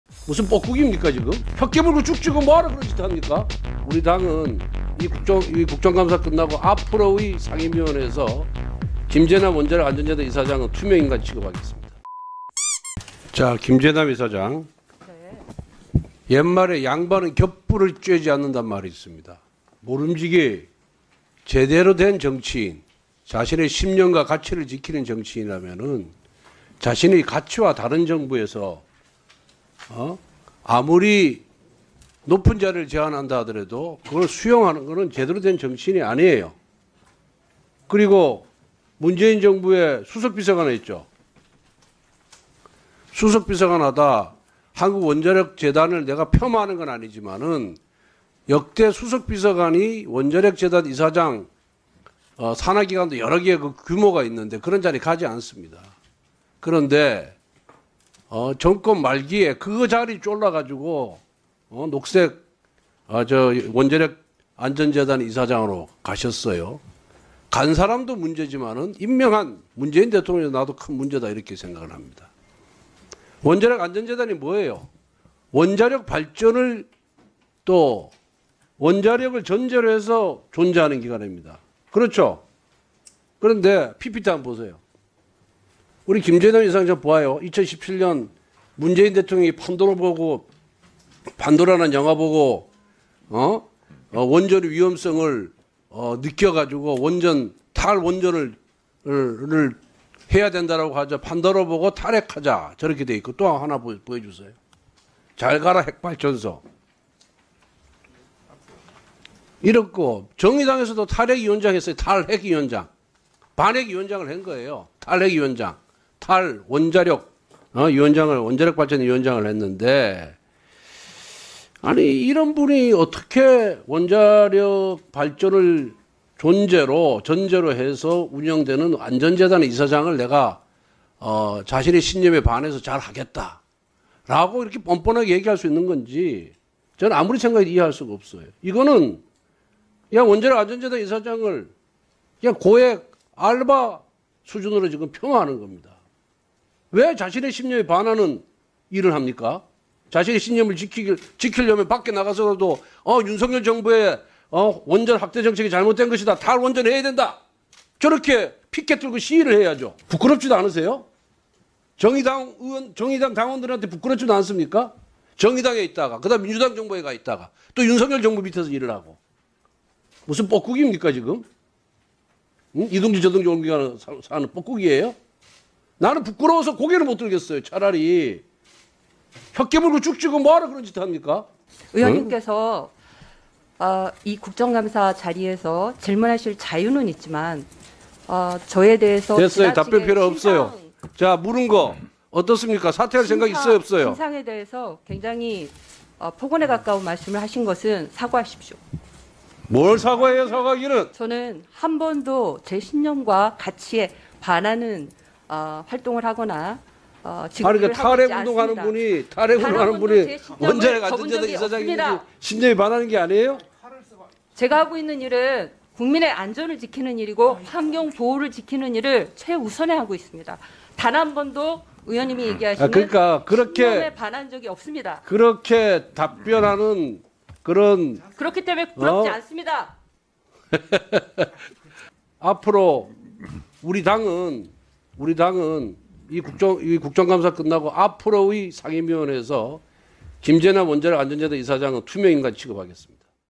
“뻐꾸기냐? 혀 깨물고 죽지” 권성동 국감서 폭언 논란…김제남 “사과하세요”
권성동 국민의힘 의원이 7일 국회 과학기술정보방송통신위원회의 원자력 관련 기관 국정감사에서 지난 정부 때 임명된 김제남 원자력안전재단 이사장에게 사퇴 압박 발언을 했다.